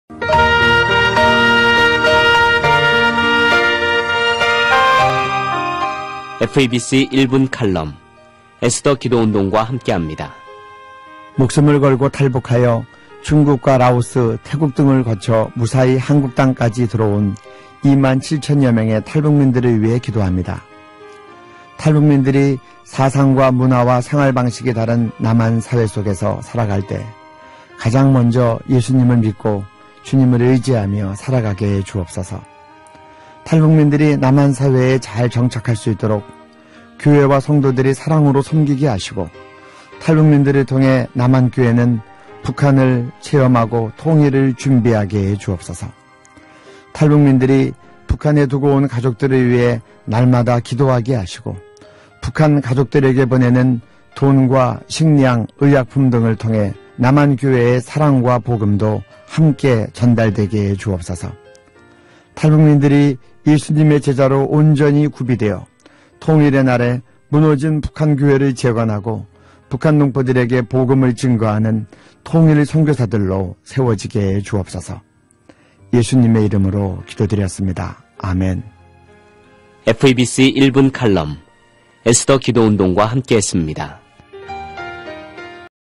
극동방송 1분기도(이용희 교수) 2015 3월 3주 탈북민
극동방송 1분칼럼 - 탈북민들을 위한 기도

목숨을 걸고 탈북하여 중국과 라오스, 태국 등을 거쳐 무사히 한국 땅까지 들어온 2만 7천 여 명의 탈북민들을 위해 기도합니다.

탈북민들이 사상과 문화와 생활방식이 다른 남한사회 속에서 살아갈 때 가장 먼저 예수님을 믿고 주님을 의지하며 살아가게 해 주옵소서.

탈북민들이 남한 사회에 잘 정착할 수 있도록 교회와 성도들이 사랑으로 섬기게 하시고 탈북민들을 통해 남한교회는 북한을 체험하고 통일을 준비하게 해 주옵소서.

탈북민들이 북한에 두고 온 가족들을 위해 날마다 기도하게 하시고 북한 가족들에게 보내는 돈과 식량, 의약품들을 통해 남한교회의 사랑과 복음도 함께 전달되게 해 주옵소서.

탈북민들이 예수님의 제자로 온전히 구비되어 통일의 날에 무너진 북한교회를 재건하고 북한 동포들에게 복음을 증거하는 통일선교사들로 세워지게 해 주옵소서.

예수님 이름으로 기도드렸습니다. 아멘